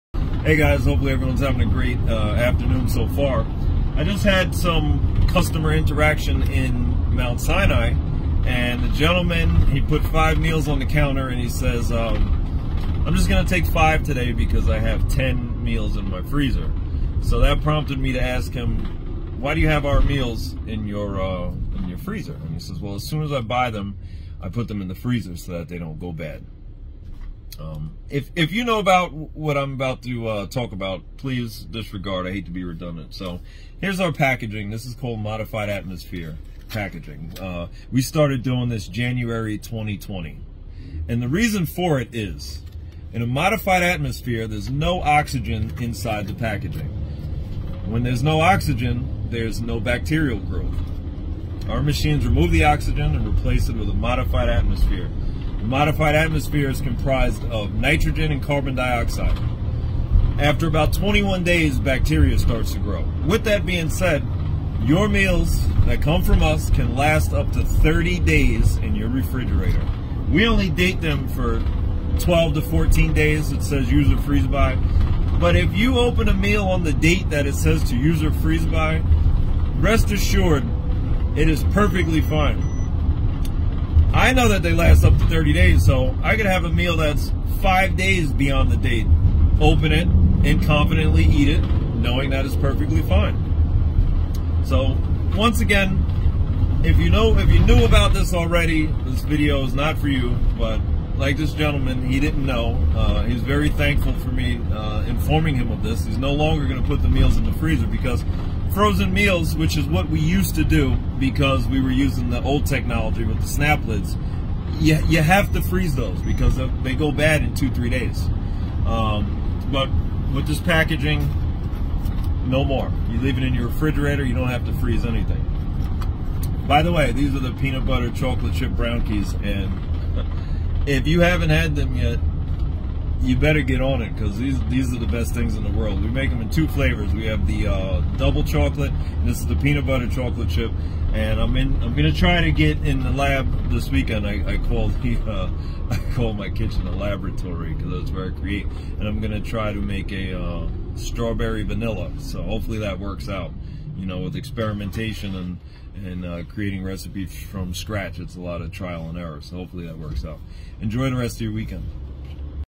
HMD Meals Last! No Need to Freeze Them With Our Packaging!
Did you realize how long our meals last in your refrigerator? No need to freeze them!  We are conservative with the dates we label our meals with.  Our packaging extends our shelf life up to 30 days.  Opening a meal on the day of expiration? Of course it's still good!